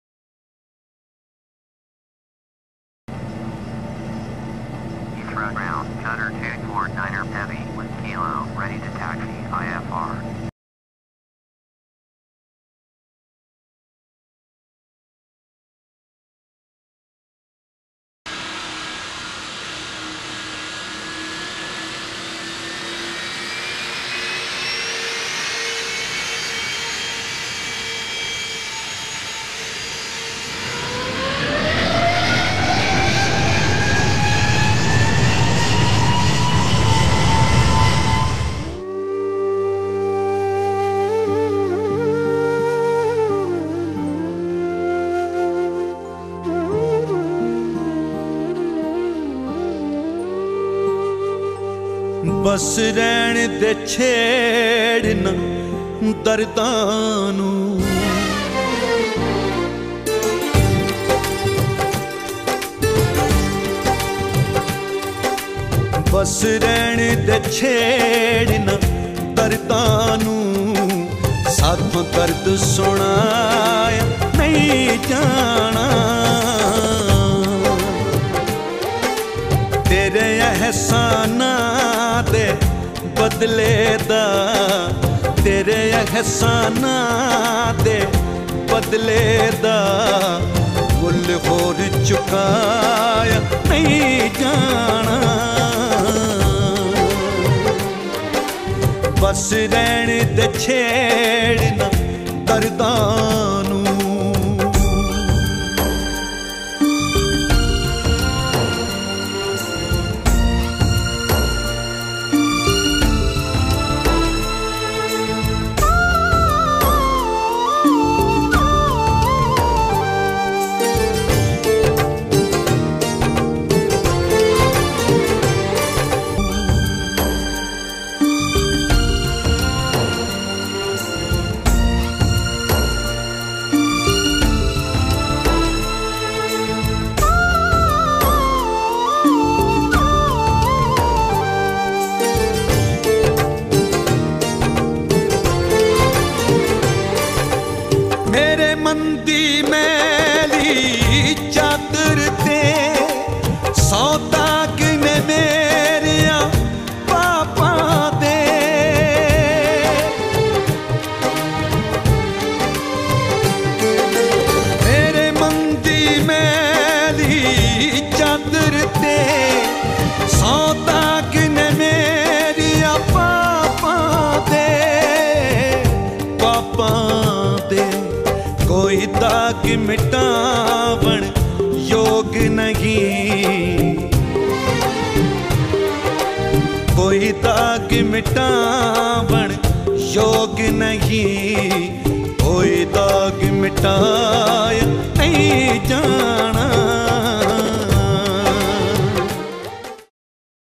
Skytail-Qatar Codeshare to Doha
Qatar Airways A330 300, flown by Imperial Captain Khan, Skytail Pilot takes to the skies on their first codeshare flight. I hope you enjoy!